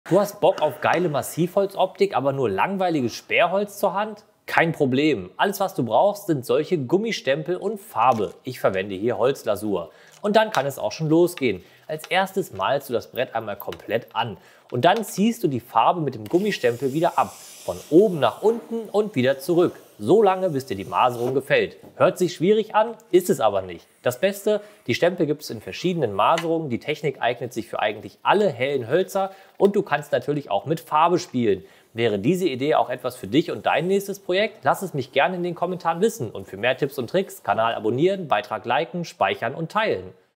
Probier DAS! | Jonas Winkler
Bock auf Massivholz-Optik, aber nur Sperrholz zur Hand? Das musst du probieren!